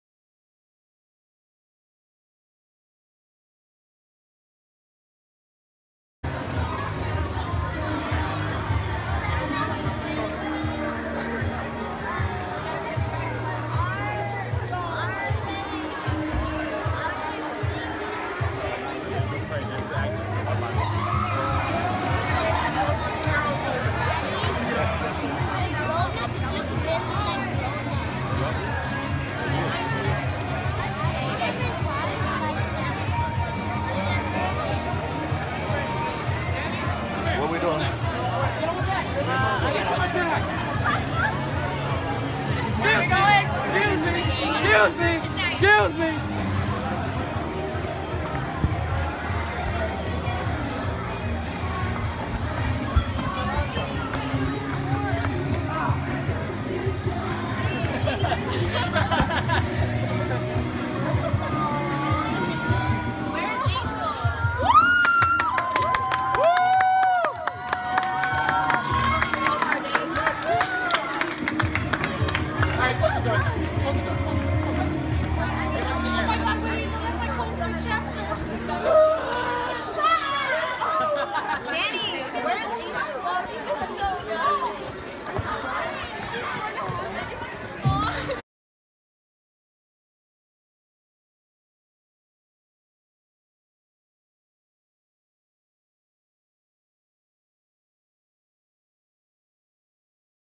Banquet Proposal for My Best Friend
my best friend means the world to me so i thought id do something that would show him that.